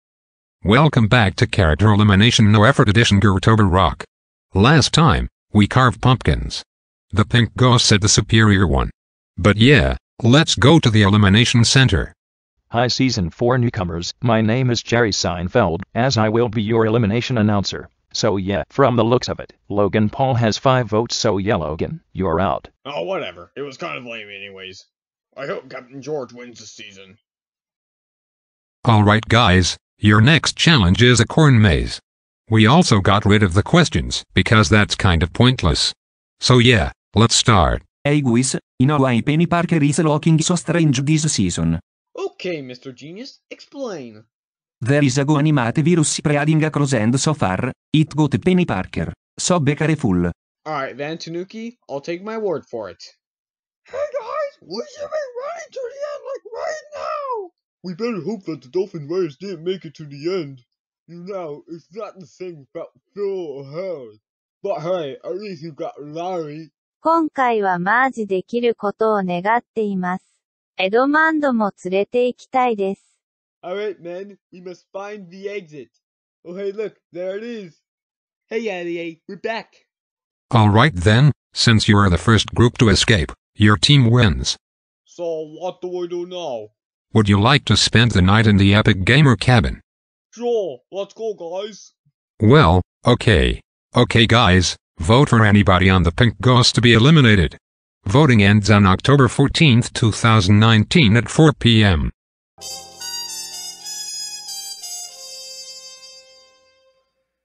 Character Elimination: No Effort Edition Goretober Rock Episode 2
It's them hopping around a Corn Maze. Nothing special.